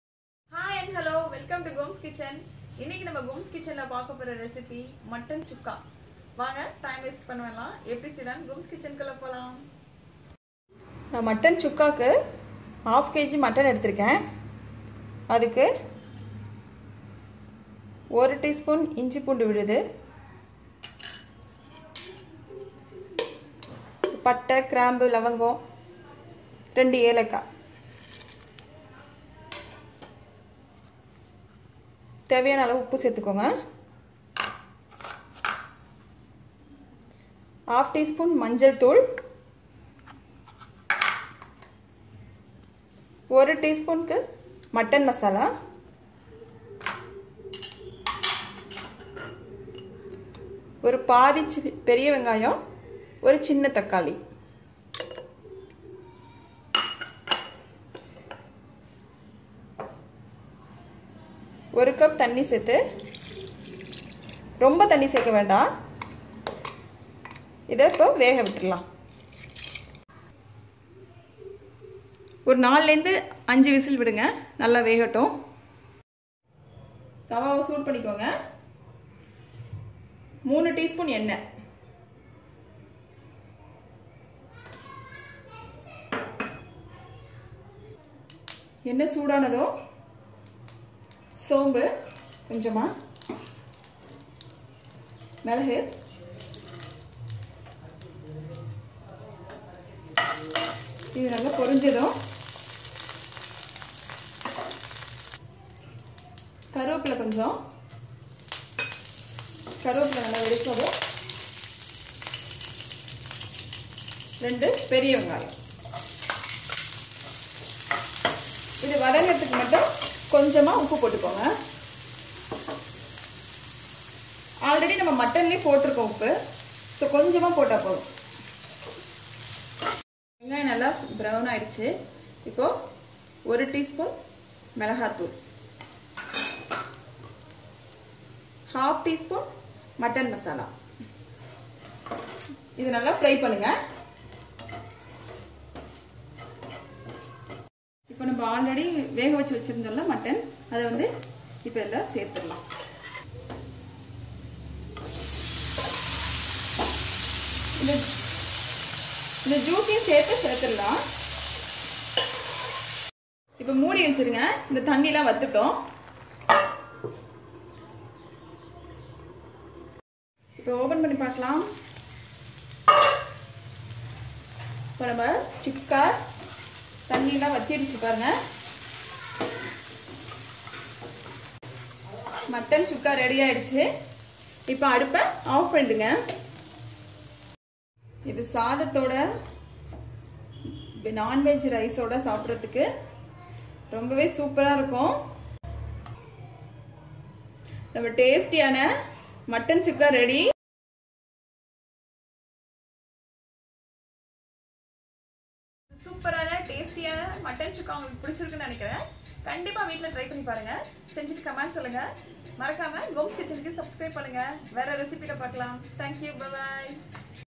மட்டன் சுக்கா வறுவல் /Mutton Chukka/Mutton Dry Fry@Gomz Kitchen
This Mutton Chukka recipe is very simple and  excellent recipes.Its a very good side dish for Rice even Non vegetarian rice. Because We are boiled the Mutton with a masala's  so mutton will be soaked for in it.So that mutton will give a very good taste.And when your cook this recipe is invite your neighbors.So please friends try this recipe soon at your home and post commend at the commend section.
Enjoy the Coking

Ingredients;
Mutton-1/2 kg
Ginger garlic paste-1 tsp
Turmeric pwd-1/2 tsp
Cardamom-3
Cloves-4
Bay leaf-2
Cinnamon-2"
Salt required
Mutton masala-1 tsp
Oil-3 tsp
Funnel seeds-1 tsp
Black pepper-1 tsp
Curry leaves
Onion-2

Blogger/Gomz Kitchen

GOMZ KITCHEN